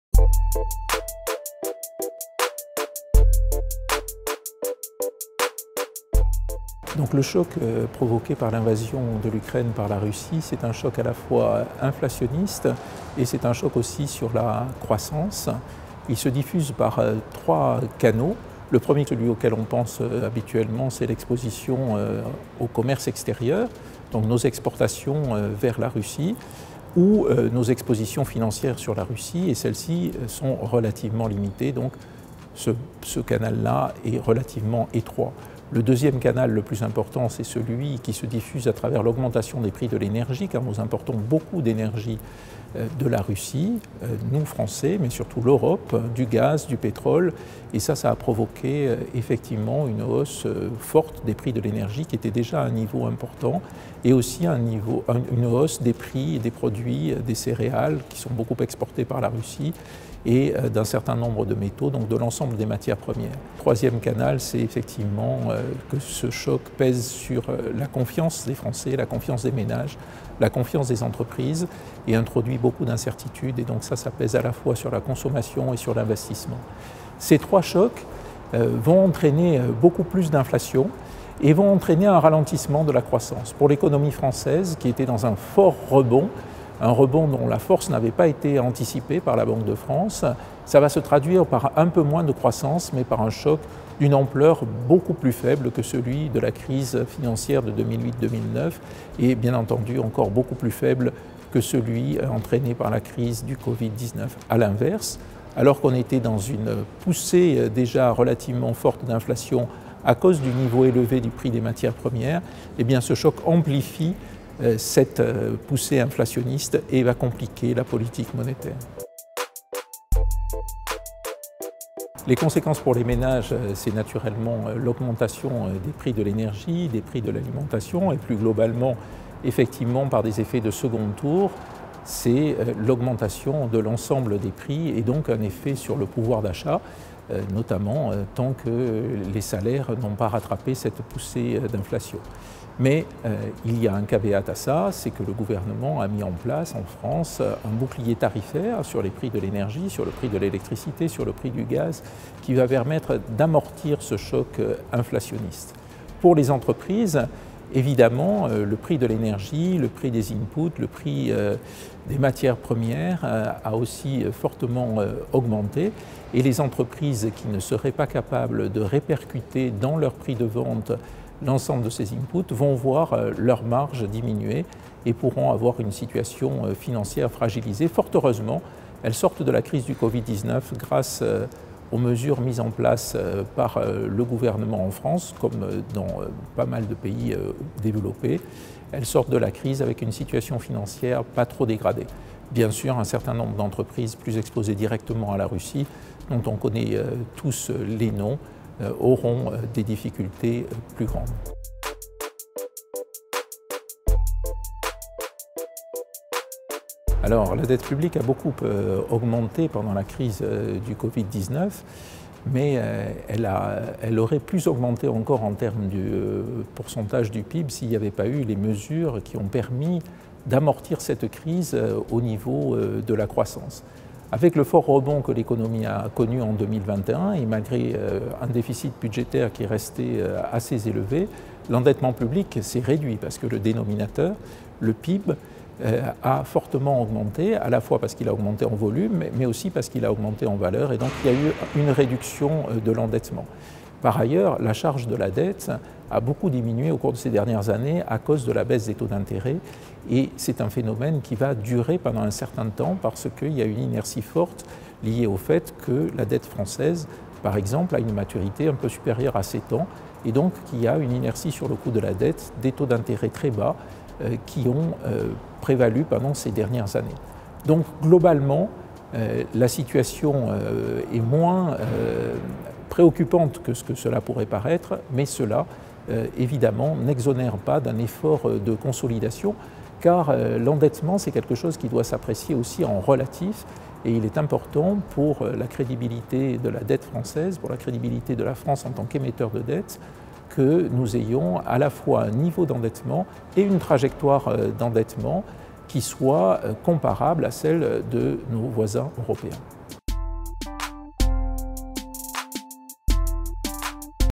Bruno CABRILLAC - (Banque de France) - RAEF 2022 - CESE
Questions à M. Bruno CABRILLAC, Directeur général adjoint des Études et des relations internationales à la Banque de France, auditionné par la commission économie et finances du CESE dans le cadre du "Rapport annuel sur l’état de la France – RAEF 2022".